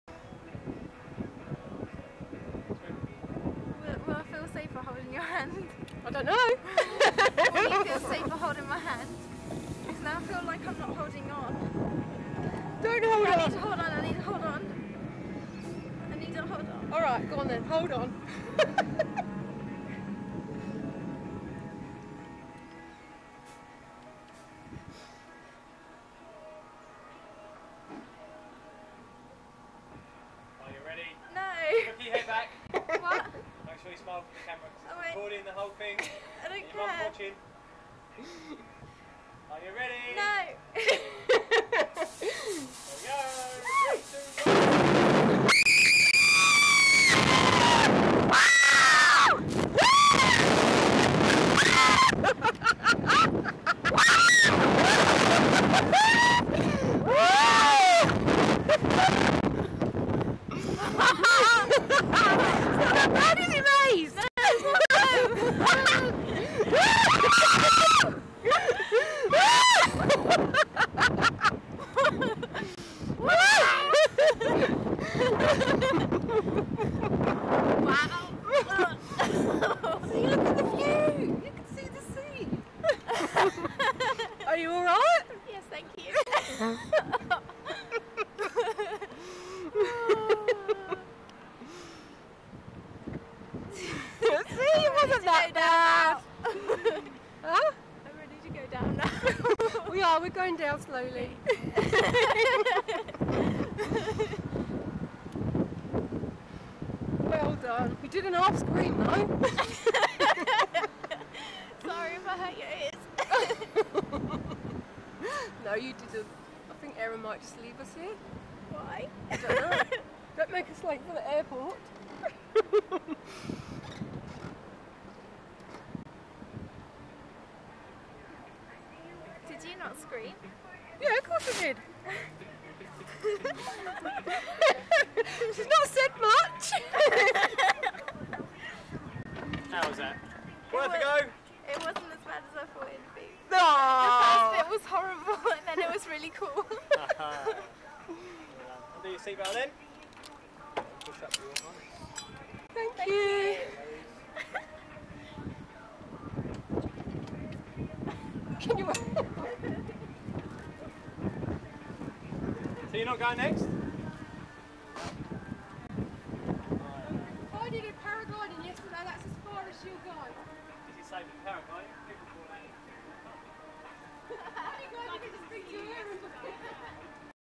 Slingshot reaction
Me and Karen on the Slingshot. The thought of it is definitely scarier than actually being on it, after the first bit..!